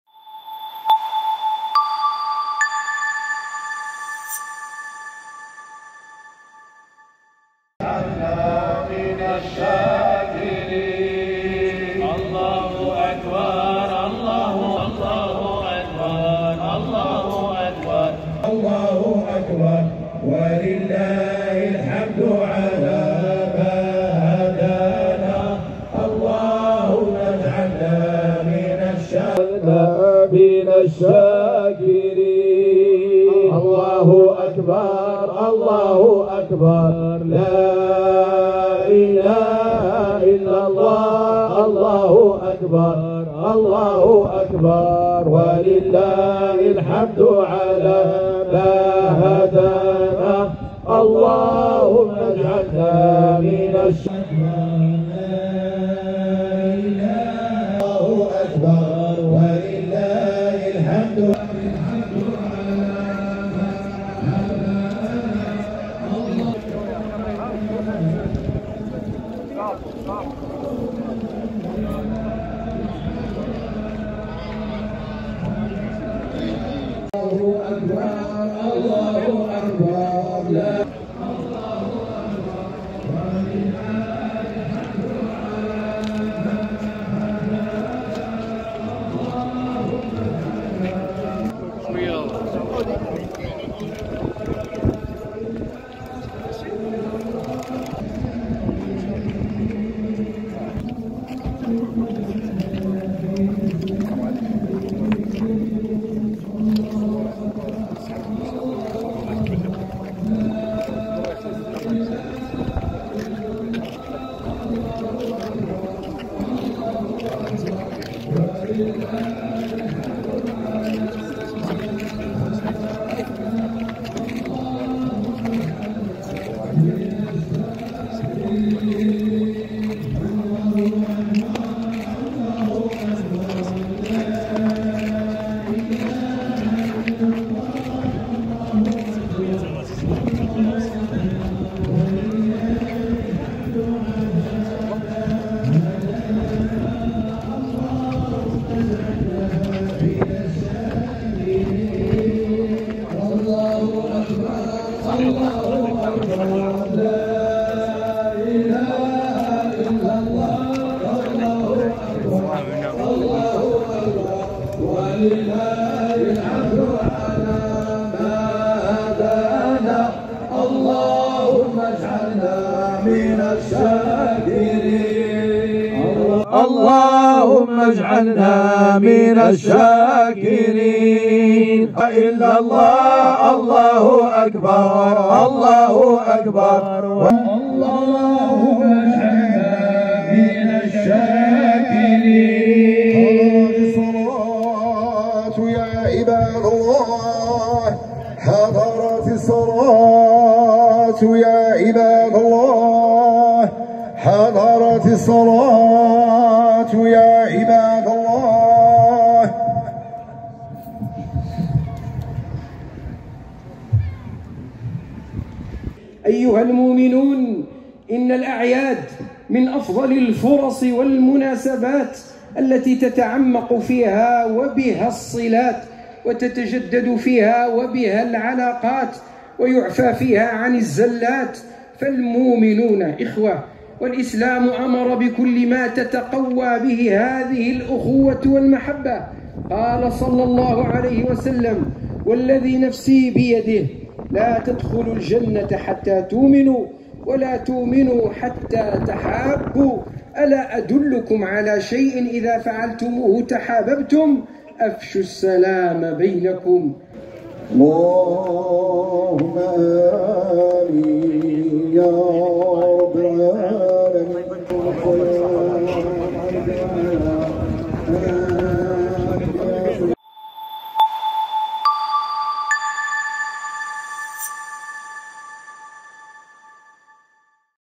صلاة العيد بطنجة .. أجواء روحانية تغمرها الفرحة والبهجة
جريدة المغرب7  .. استقلالية، مسؤولية وموضوعية